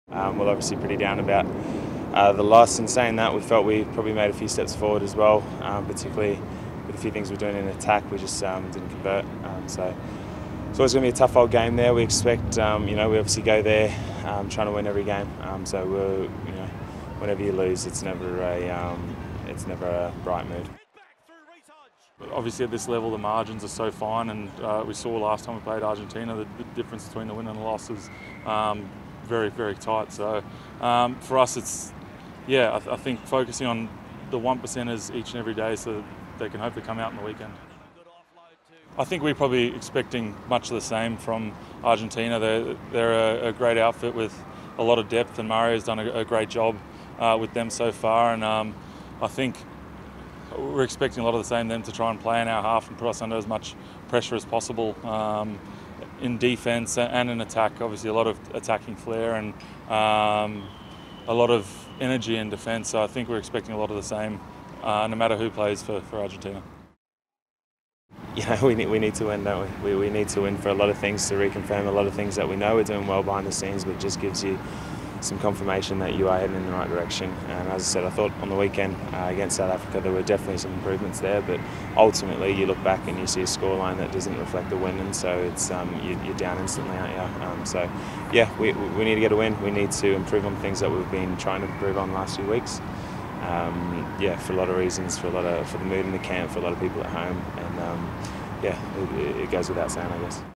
One mission as Wallabies arrive in Argentina
The Wallabies have arrived in Buenos Aires ahead of their showdown with the Pumas in Salta on Sunday morning (AEST).  Backs Matt Toomua and Dane Haylett-Petty with one mission in mind for their final TRC game of 2018.